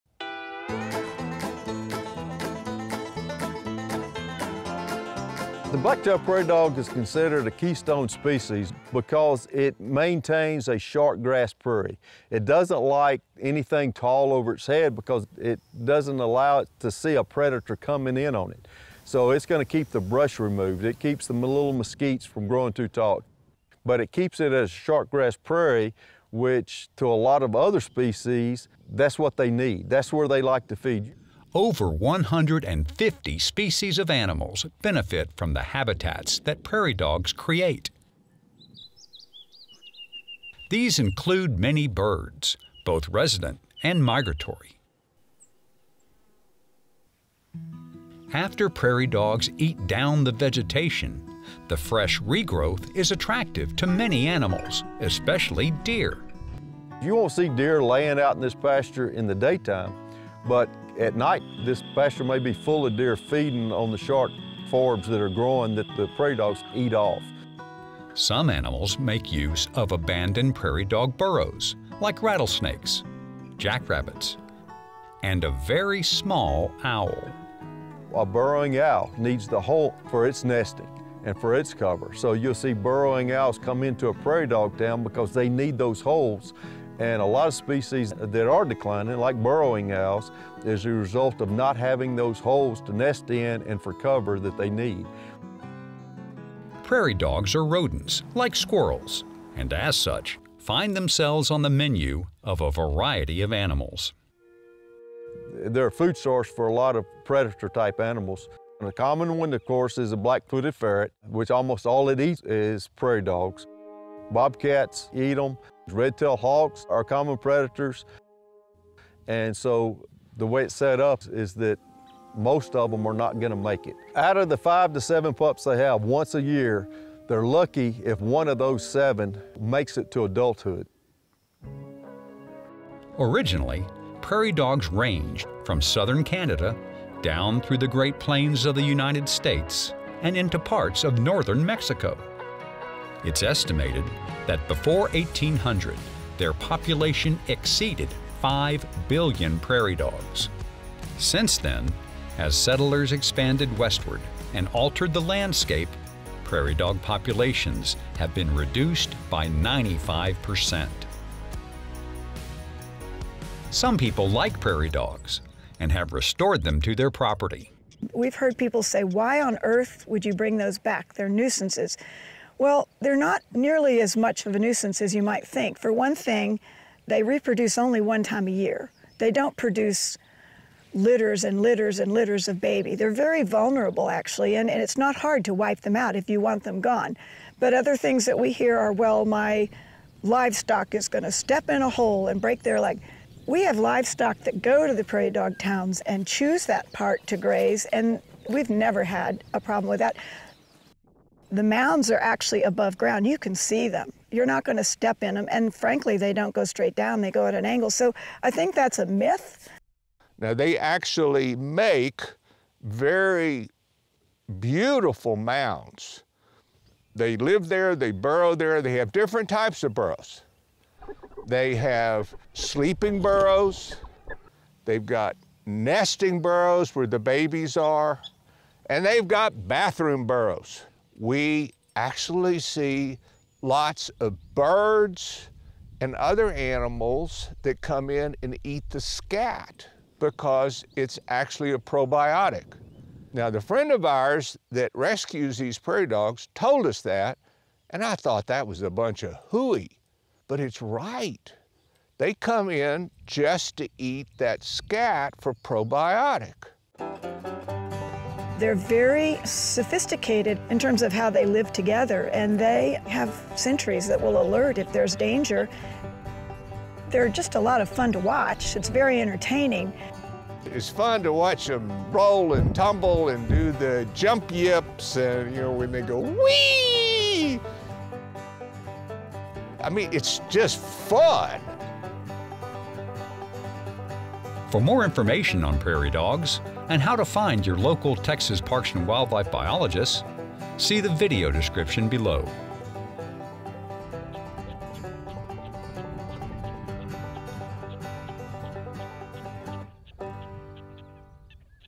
Prairie Dogs: Ecosystem Engineers
Prairie dog are a keystone species that impact over 150 different species of animals. They create habitats that various animals use and they are prey for several others.

00:00- introduction
00:04- prairie dogs create short grass prairies
00:26- other animals use habitat created
00:32- many birds use habitat
00:47- regrowth attracts deer
01:08- some animals use prairie dog burrows
01:17- burrowing owls nest in prairie dog burrows
01:36- prairie dogs are prey
01:49- black-footed ferret
02:12- historical range and population
02:30- decline
02:43- restored prairie dogs
02:50- not a nuisance
03:34- different burrows
03:38- scat is probiotic for other animals
04:20- fascinating and fun